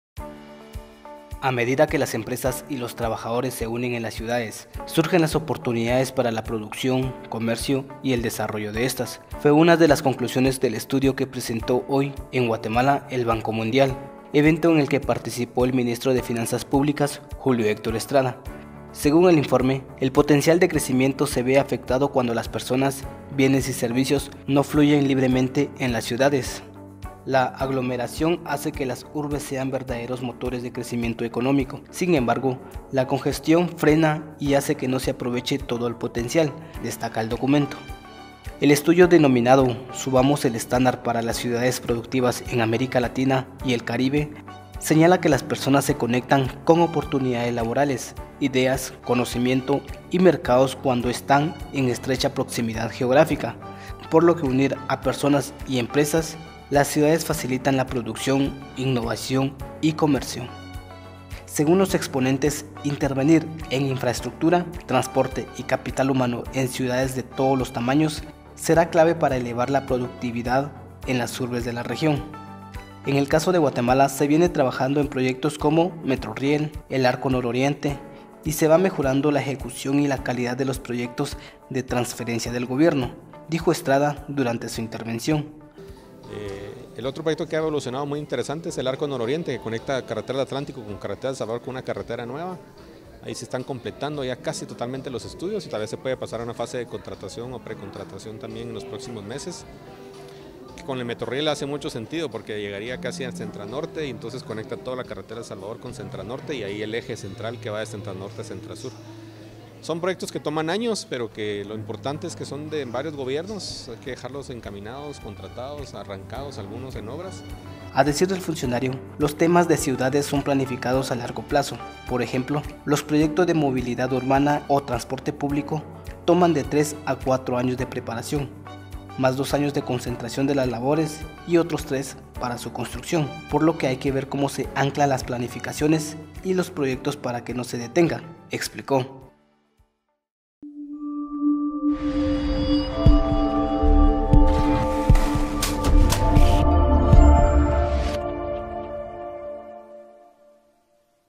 Banco Mundial presenta estudio sobre ciudades productivas en América Latina y el Caribe
A medida que las empresas y los trabajadores se unen en las ciudades, surgen las oportunidades para la producción, comercio y el desarrollo de estas, fue unas de las conclusiones del estudio que presentó hoy en Guatemala el Banco Mundial, evento en el que participó el Ministro de Finanzas Públicas, Julio Héctor Estrada.

Según el informe, el potencial de crecimiento se ve afectado cuando las personas, bienes y servicios no fluyen libremente entre las ciudades.

“La aglomeración hace que las urbes sean verdaderos motores de crecimiento económico. Sin embargo, la congestión frena y hace que no se aproveche todo el potencial”, destaca el documento.

El estudio, denominado “Subamos el estándar para las ciudades productivas en América Latina y el Caribe”, señala que las personas se conectan con oportunidades laborales, ideas, conocimiento y mercados cuando están en estrecha proximidad geográfica, por lo que “al reunir a personas y empresas en estrecha proximidad geográfica, las ciudades facilitan la producción, innovación y comercio”.

Invertir en transporte y capital humano, la clave

Según los exponentes, invertir en infraestructura, transporte y capital humano en ciudades de todos los tamaños será clave para elevar la productividades en las urbes de la región.

“En el caso de Guatemala, se viene trabajando en proyectos como Metro Riel, el Arco Nororiente y se va mejorando la ejecución y la calidad de los proyectos de transferencias del gobierno”, dijo Estrada durante su intervención.

A decir del funcionario, los temas de ciudades son planificados a largo plazo, por ejemplo, “los proyectos de movilidad urbana o transporte público toman de 3 a 4 años de preparación, más 2 años de contratación de las labores y otros 3 para su construcción, por lo que hay que ver como se anclan las planificaciones y los proyectos para que no se detengan”, explicó.